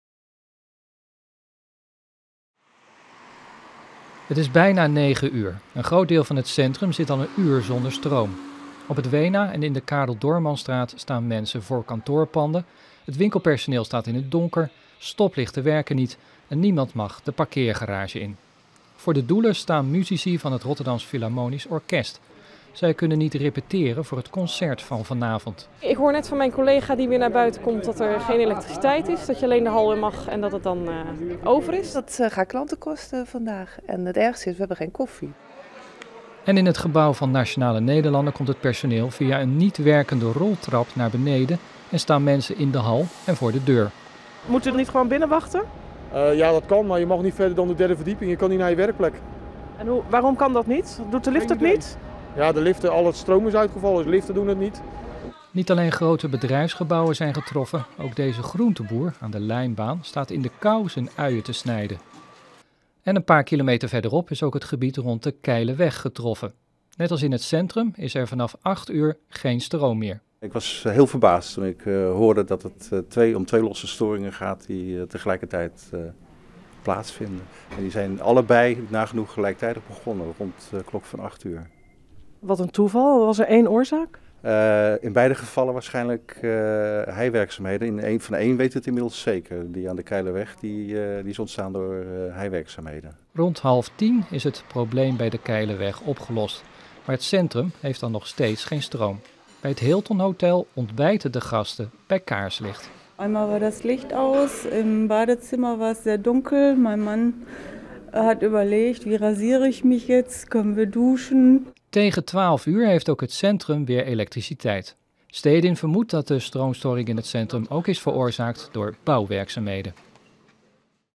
Centrum Rotterdam uren zonder stroom
Het centryum van Rotterdam heeft vrijdag een paar uur zonder stroom gezeten. Rond acht uur viel de elektriciteit uit in de omgeving van het Weena en de Keileweg. Oorzaak waren heiwerkzaamheden in het noorden van het Rotterdamse centrum.